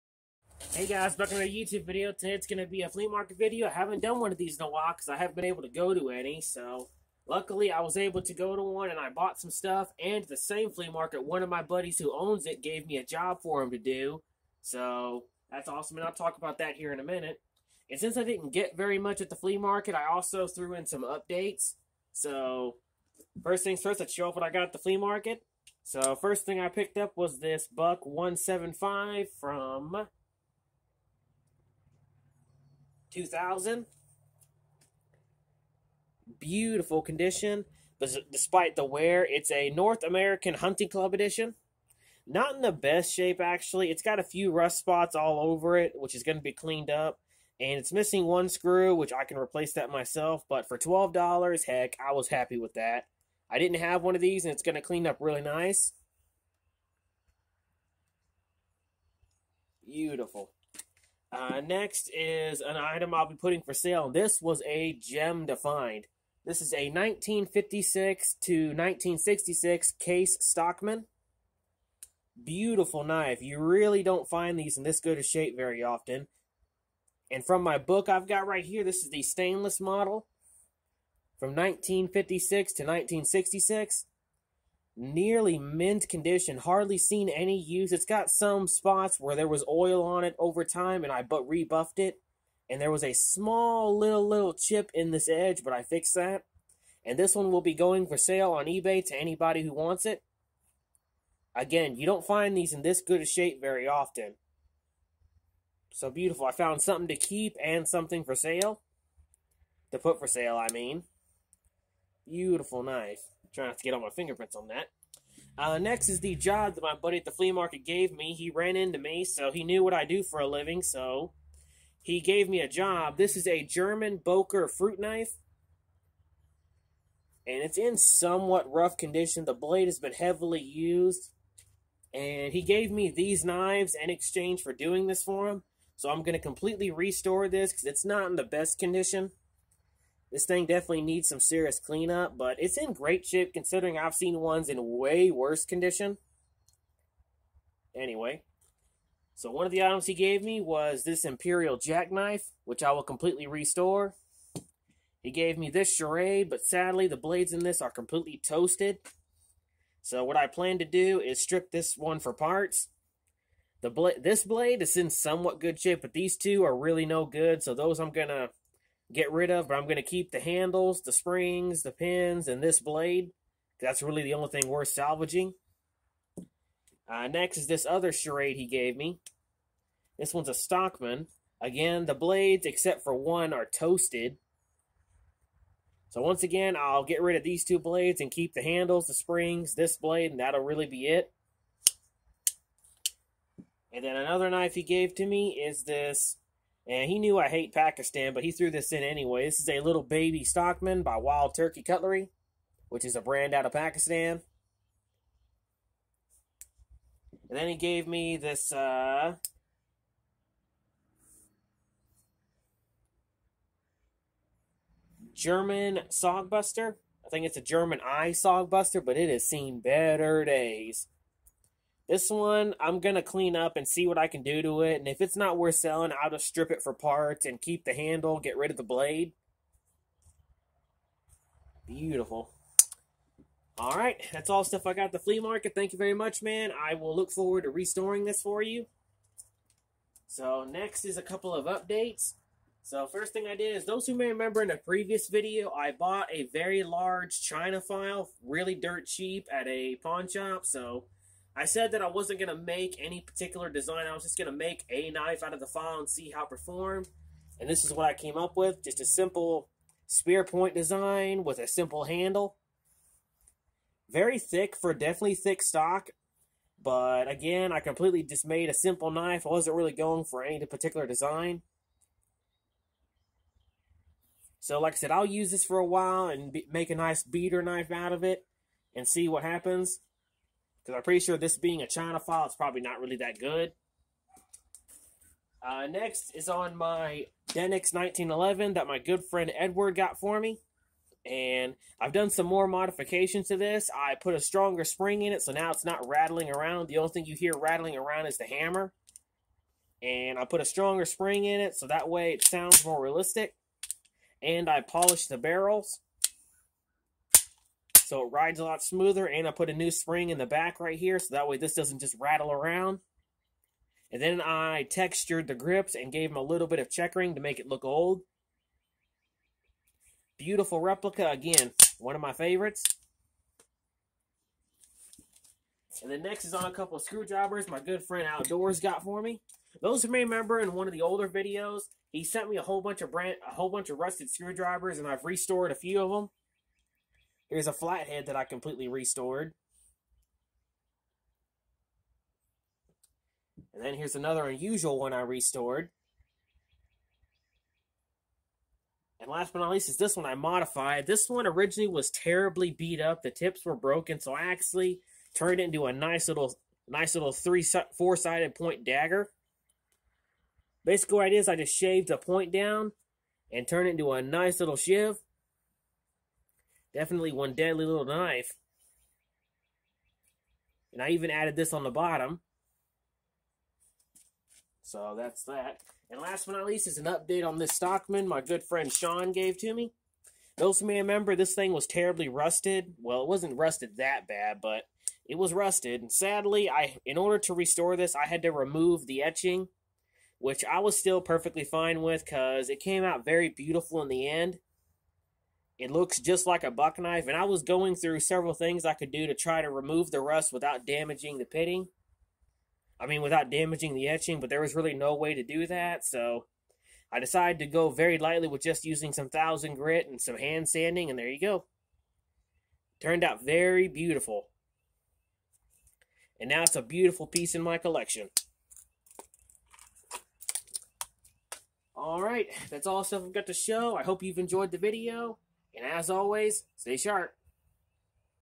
showing off some items I got at the flea market and some updates on a few items hope you enjoy
got some of these items at a flea market and since there wasn't really many to a video on I decide to throw in some update I hope you enjoy the video